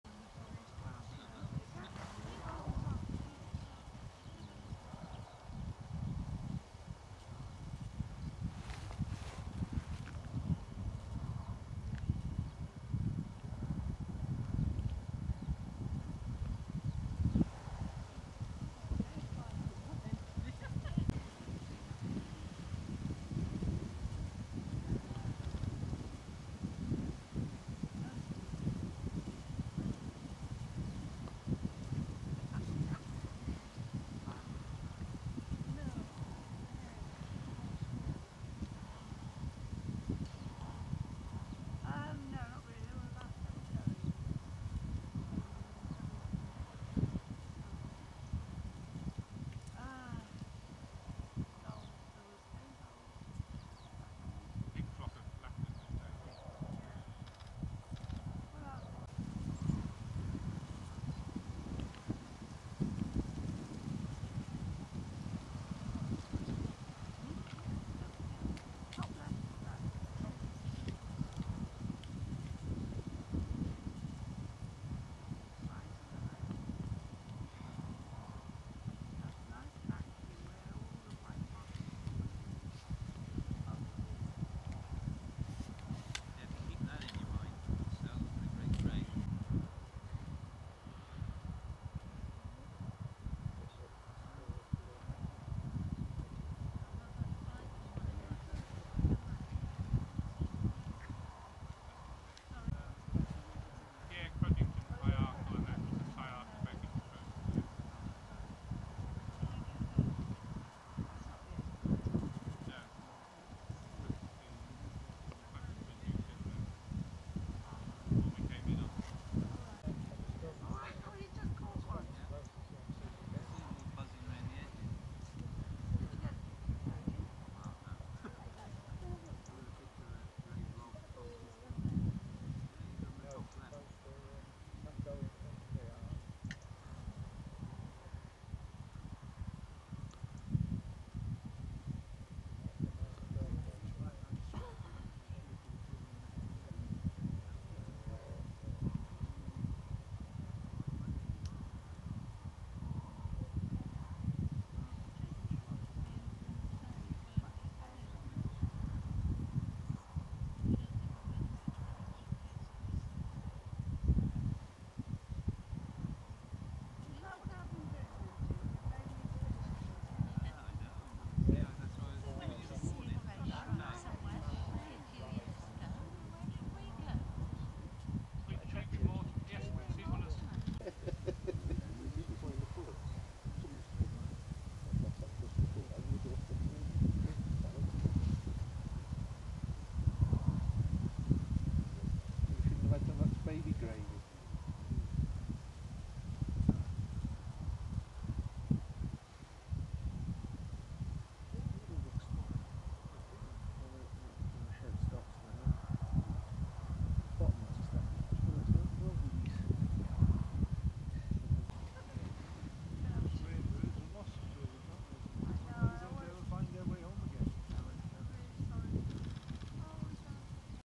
Steppe Grey Shrike video No. 2
Steppe Grey Shrike filmed at Wall farm near Telford, Shropshire on the 6th of November in sunshine. The bird was 'distant' and I had to film it using digital zoom.